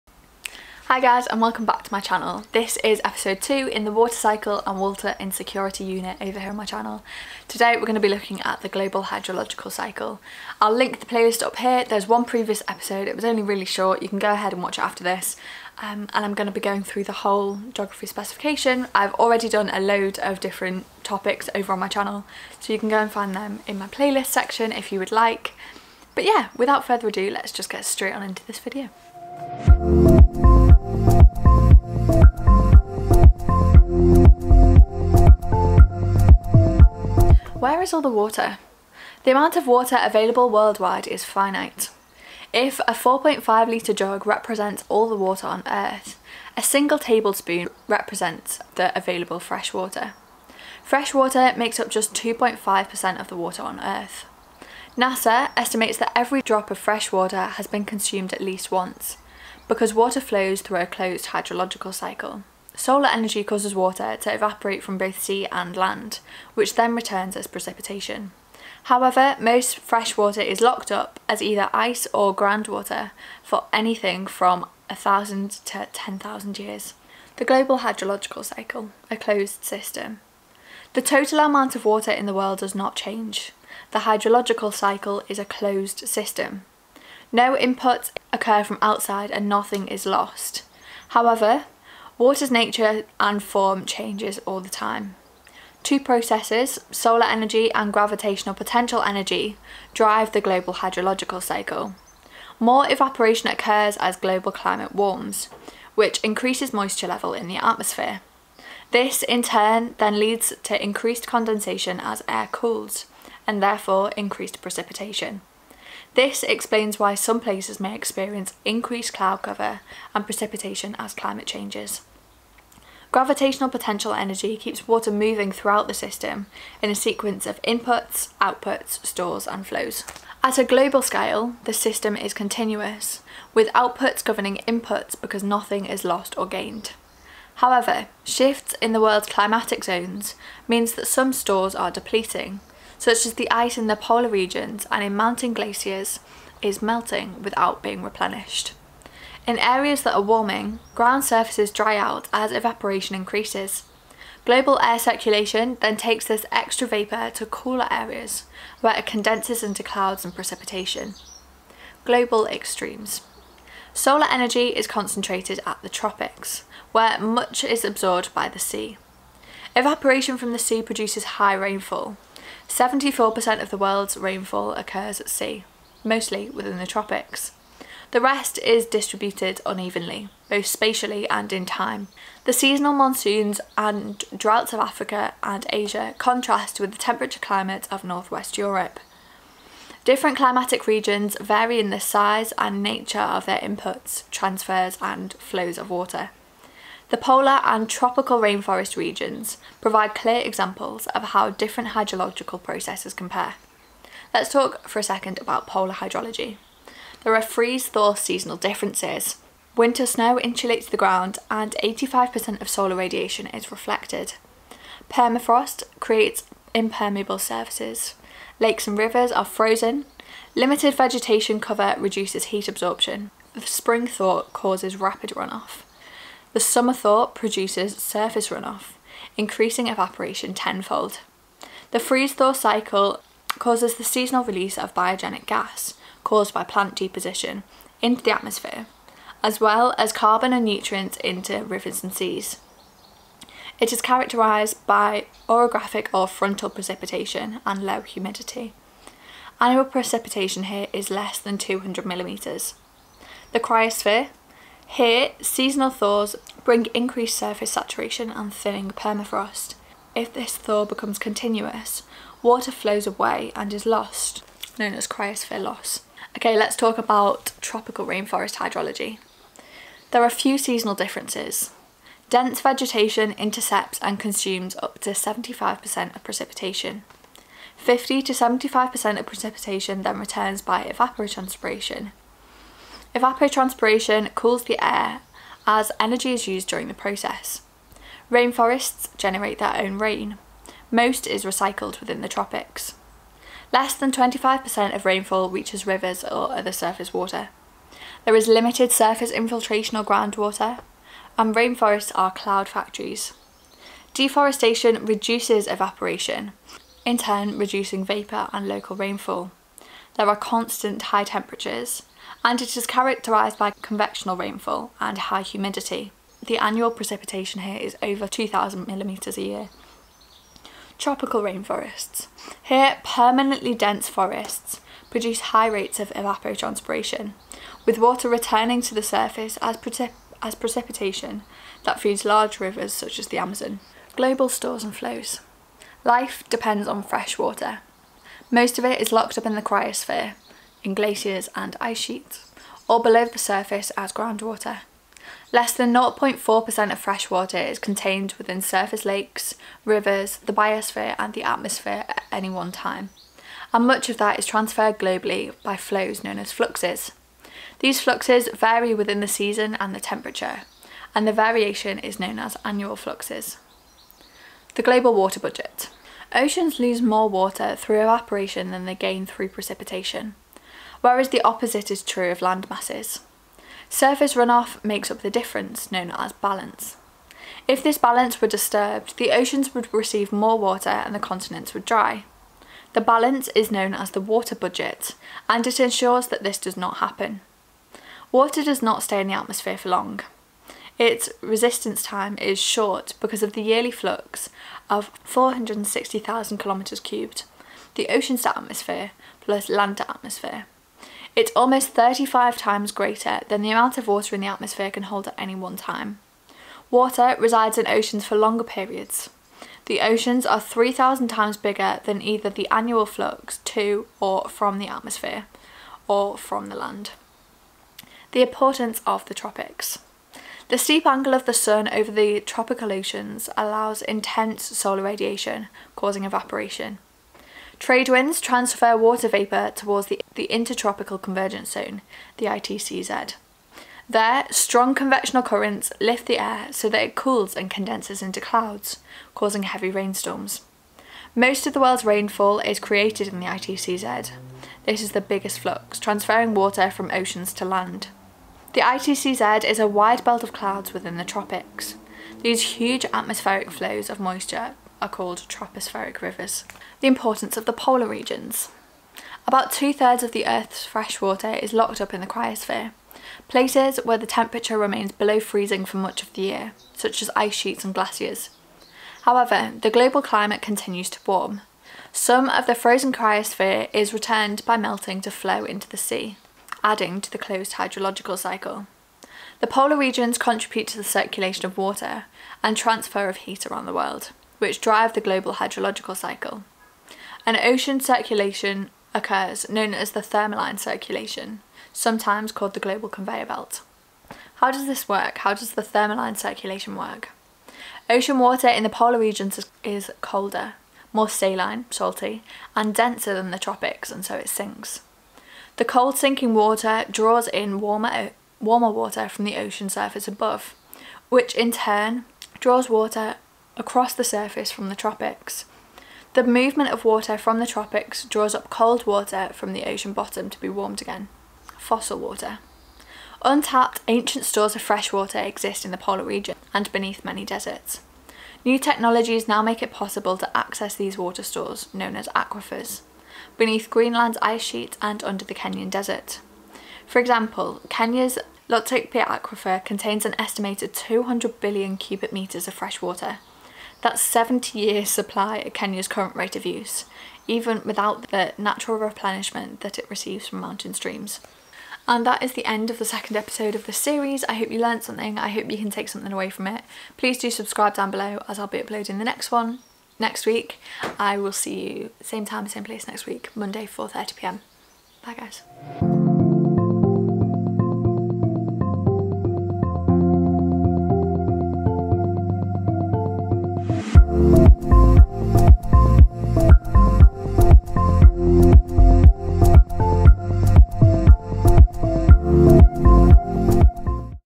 THE GLOBAL HYDROLOGICAL CYCLE | Water Cycle Revision Series #2 | A Level Geography - Edexcel, AQA
In today's video we are going to be looking at the importance of the global hydrological cycle to life on earth.

✨Equipment used✨:
* Canon 200D with 18-55mm lense
* Canon g7x Mark II
* Rode Microphone